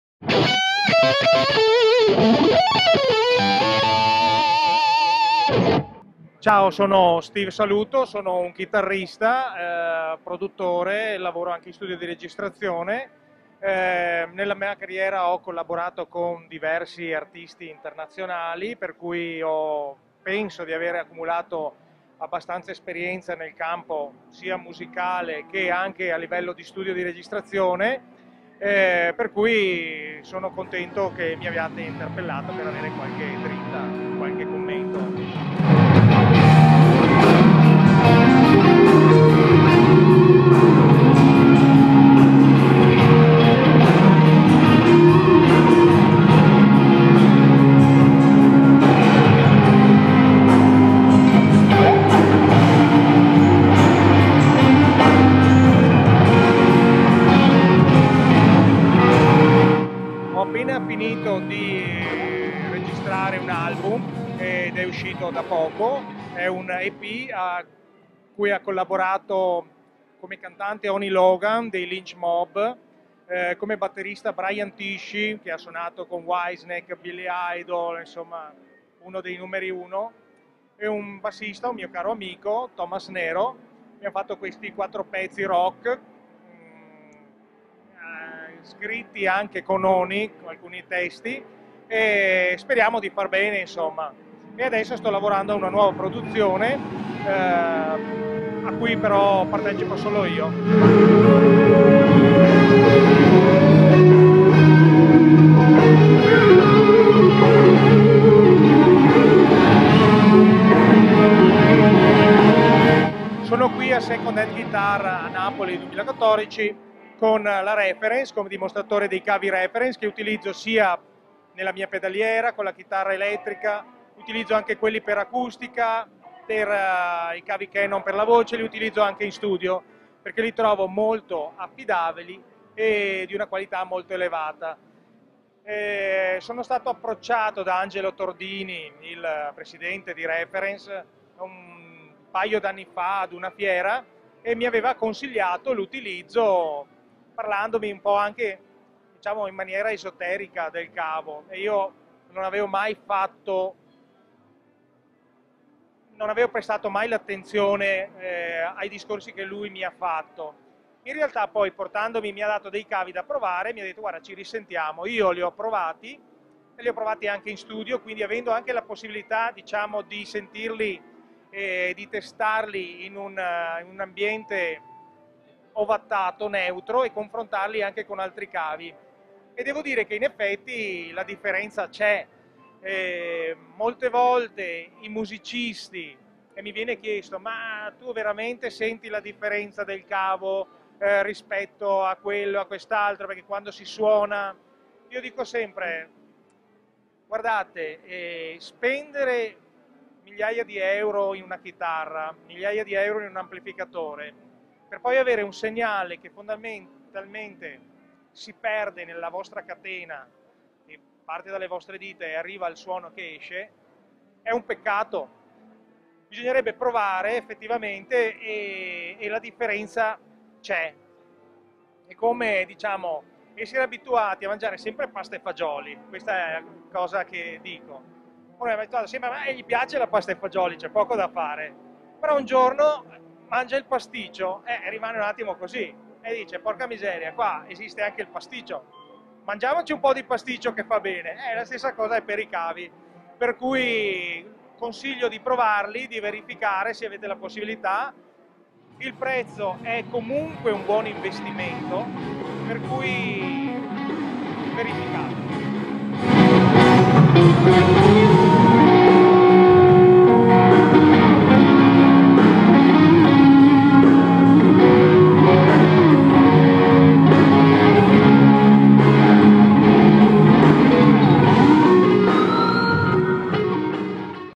SHG Napoli 2014 - Steve Saluto
Il 12 e il 13 aprile 2014 Second Hand Guitars arriva a Napoli. La Mostra d'Oltremare si riempie di chitarre, bassi, amplificatori, accessori, effetti, eventi, dimostrazioni, clinic e workshop _______________